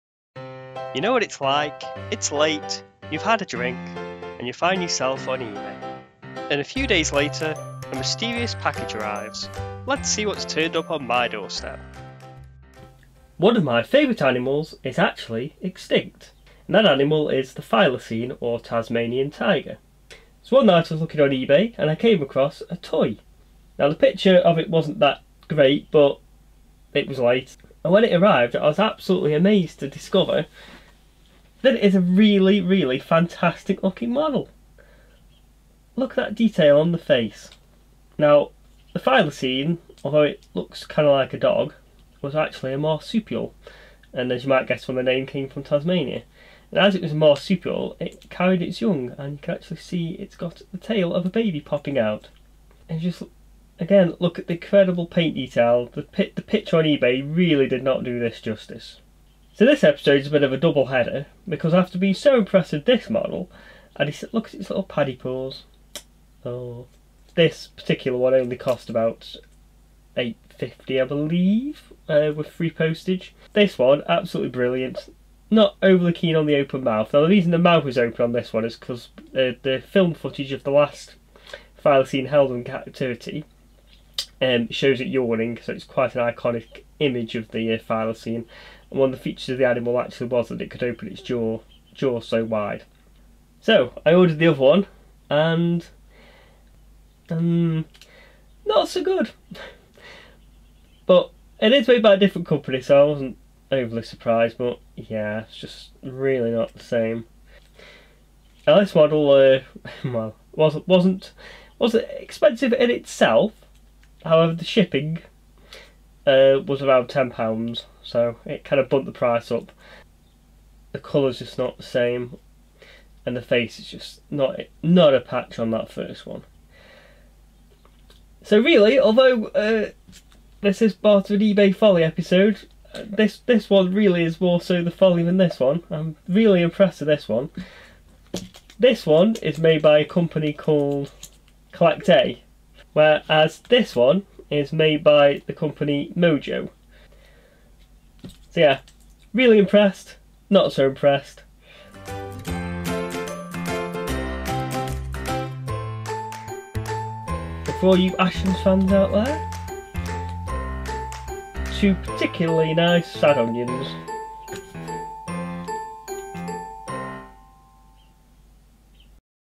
eBay Folly: Thylacine Models
The first episode of the series, where Mike reviews items he's brought on eBay usually after a few drinks. This first episode features models of the extinct Thylacine, one from CollectA and another from Mojo.